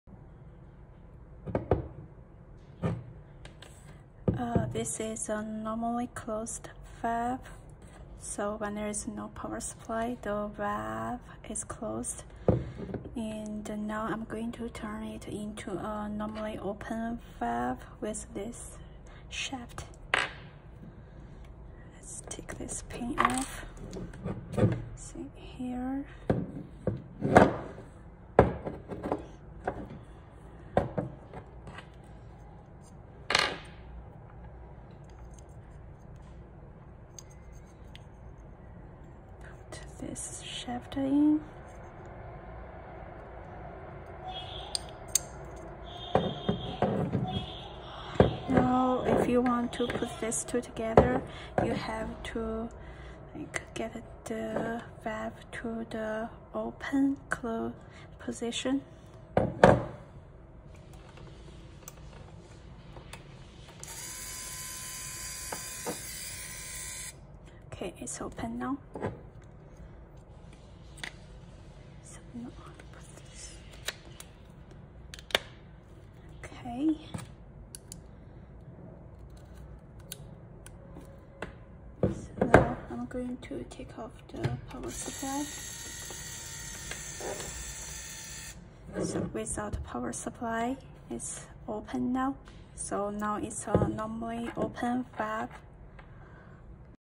How to transform a normally closed (NC) motorized ball valve into a normally open (NO) valve?
With this quick assembly design, using a slightly different connecting shaft, the motorized ball valve normally closed could be transfromed to a normally open (NO) one within minutes.